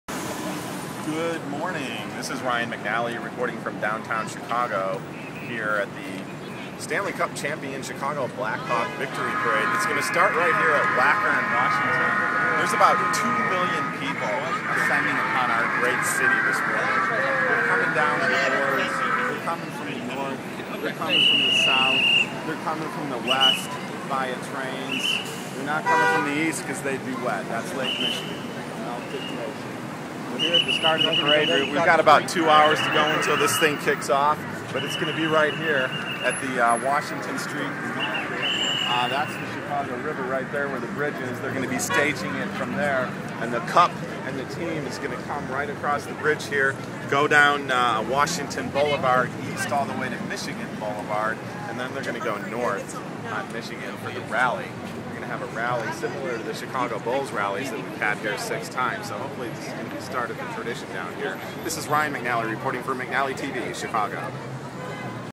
Blackhawks parade
Ryan reports from the Cup victory parade route